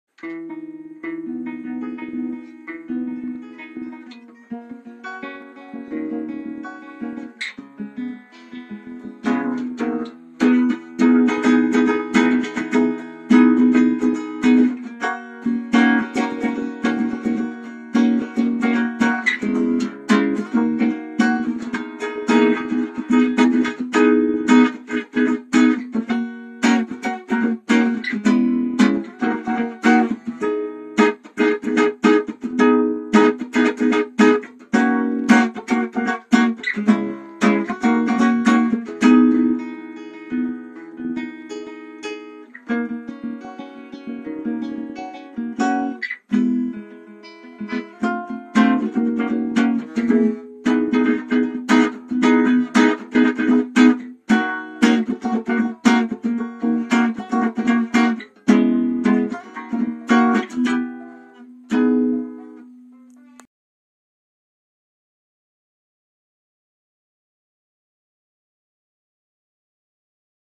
Acoustic Free Play
Friend plays riff he came up with on acoustic guitar.